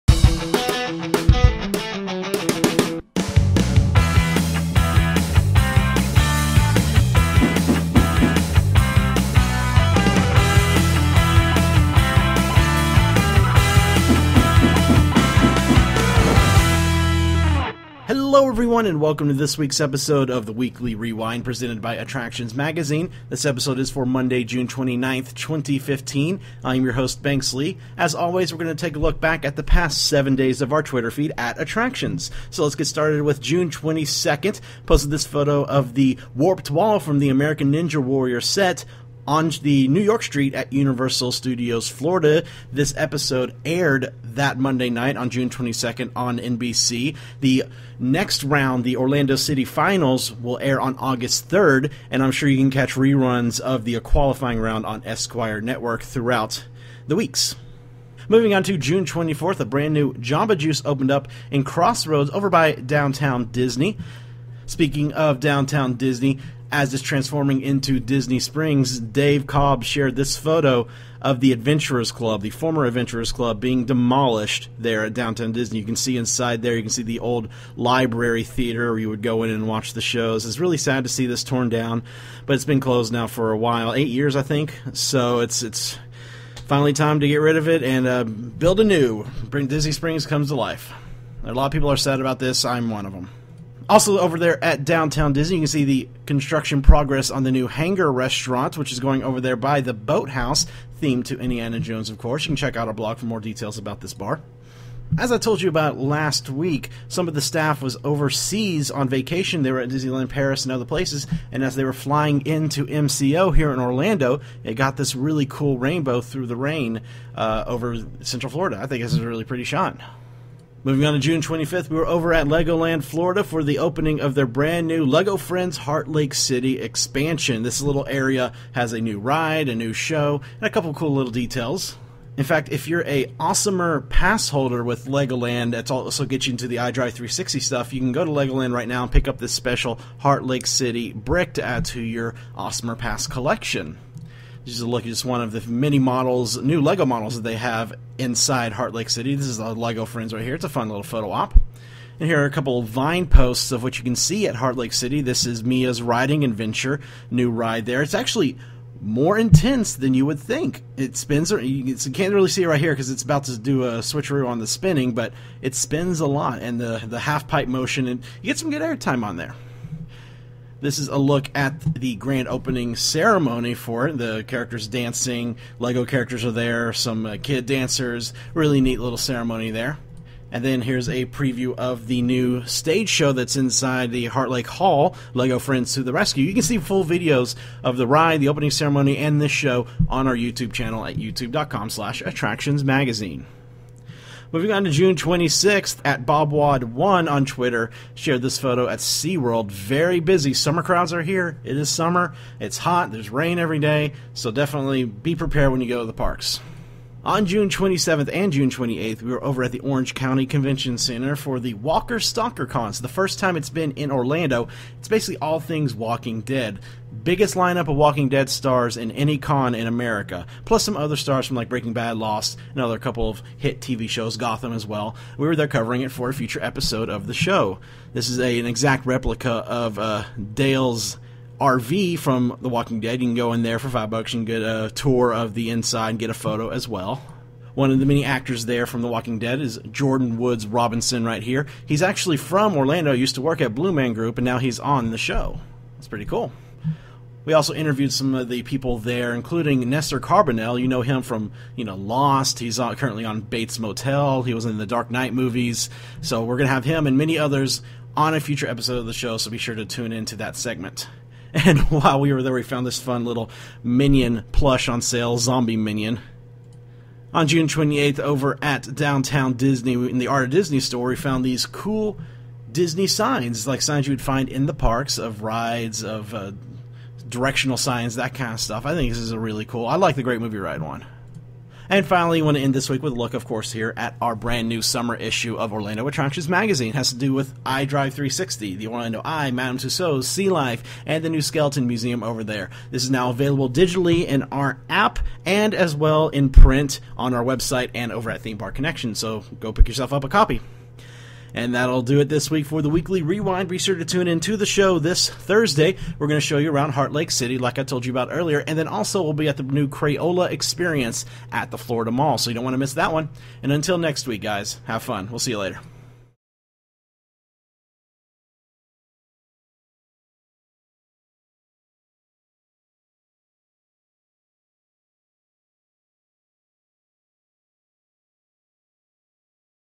The Weekly Rewind @Attractions - Heartlake City, Walker Stalker Con - June 29, 2015
A look back at the week of Tweeted photos from @Attractions Twitter feed - June 22 to June 28, 2015.